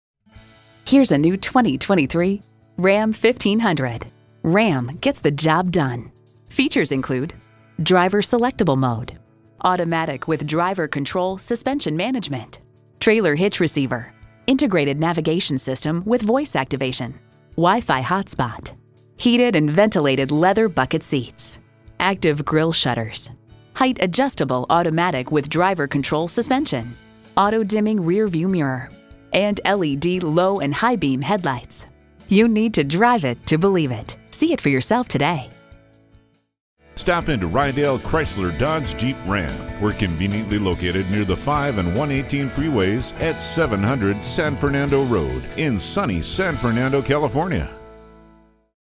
New 2023 Ram 1500 San Fernando CA Valencia, CA #608488
Year: 2023
Make: Ram
Model: 1500
Trim: LIMITED CREW CAB 4X2 5'7 BOX
Engine: 5.7L V8 HEMI MDS VVT Engine
Transmission: 8-Speed Automatic 8HP75 Transmission
Color: Granite Crystal
Interior: Black
Stock #: 608488
VIN: 1C6RREHT8PN608488
You won't want to miss this excellent value! It delivers style and power in a single package! Top features include front bucket seats, heated and ventilated seats - both front and rear, a bedliner, and voice activated navigation. Smooth gearshifts are achieved thanks to the powerful 8 cylinder engine, and load leveling rear suspension maintains a comfortable ride. Well tuned suspension and stability control deliver a spirited, yet composed, ride and drive A test drive is waiting for you.

Address:
700 San Fernando Rd.